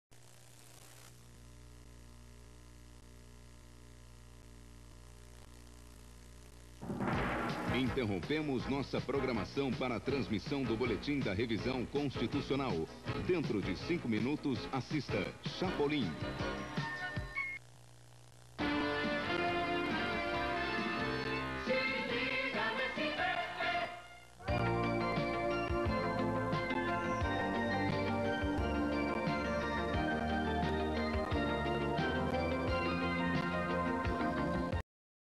Vinhetas - SBT (1994)
Algumas vinhetas clássicas do SBT em 1994, na época do Se Liga.
A vinheta infantil era a hora boa do dia :)

Obs: Essa revisão constitucional foi um fiasco.